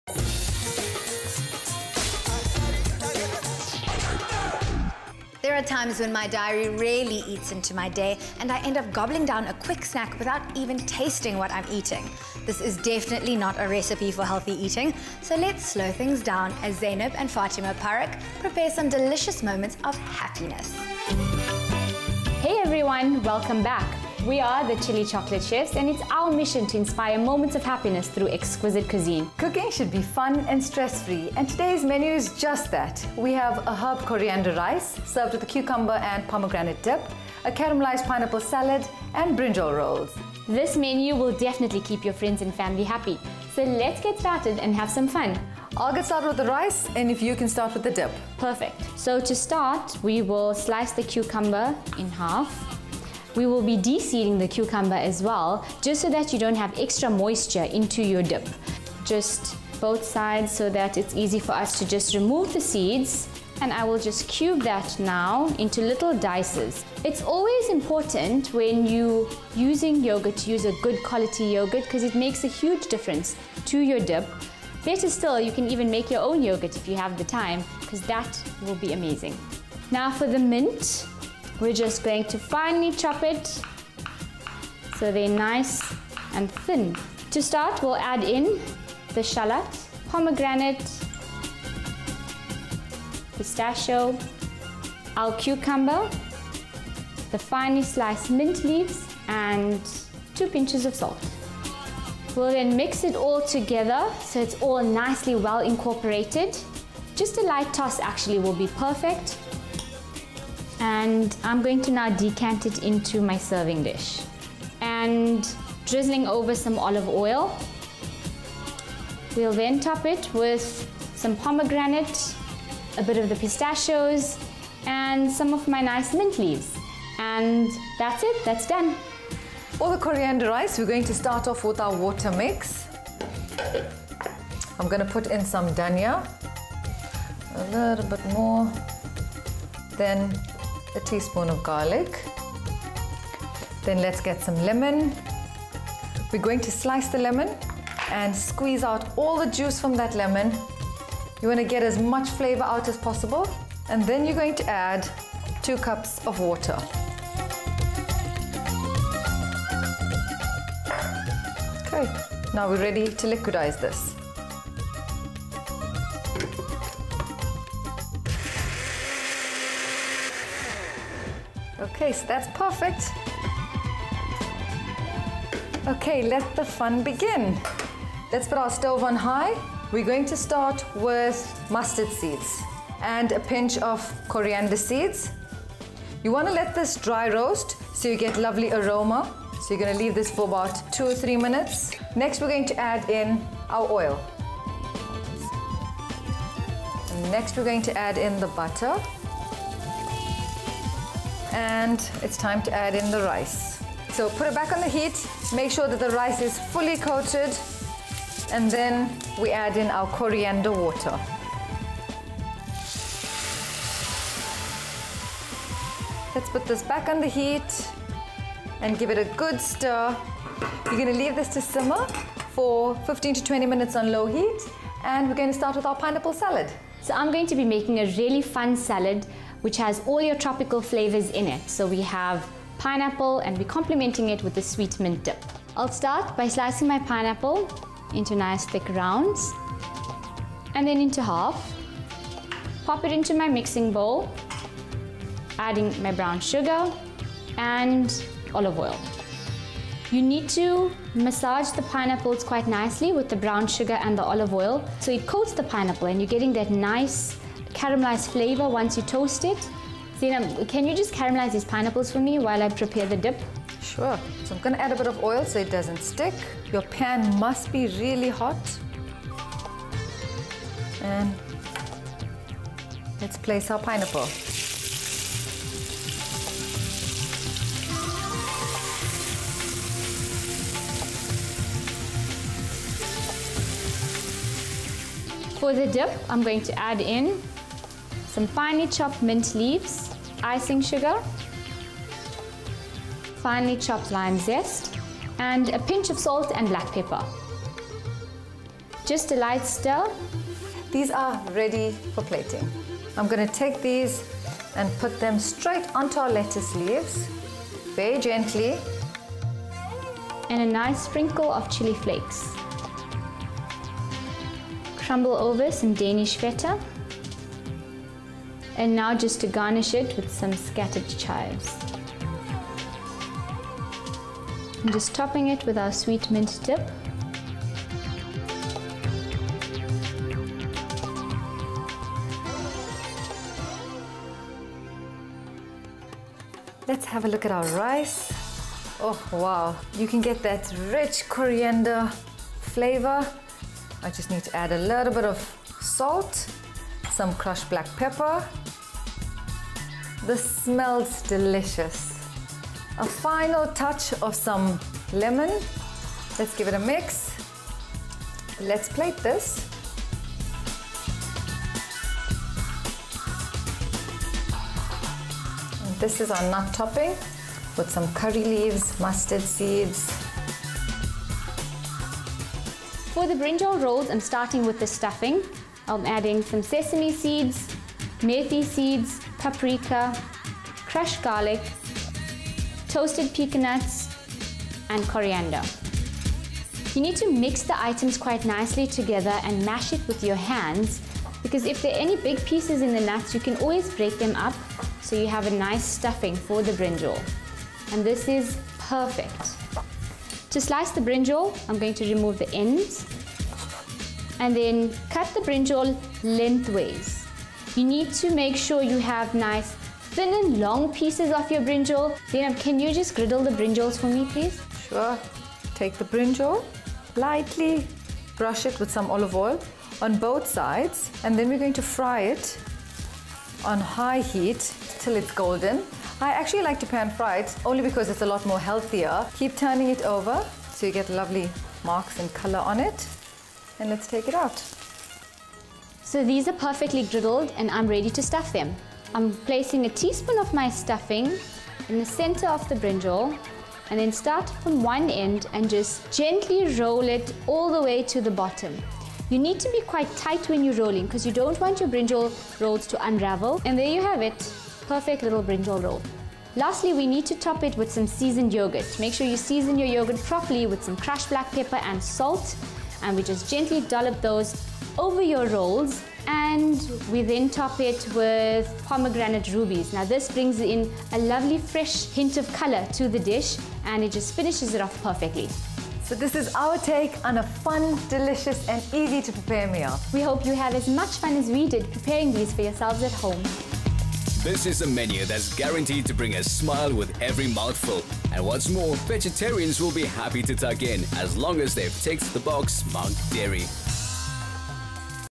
Food Mela : Faatimah and Zainab Paruks' "Moments of Happiness" Menu
The Chilli Chocolate Chefs have prepared pure pleasure for your palate with their coriander rice served with cucumber and pomegranate dip, along with caramelised pineapple salad and brinjal rolls.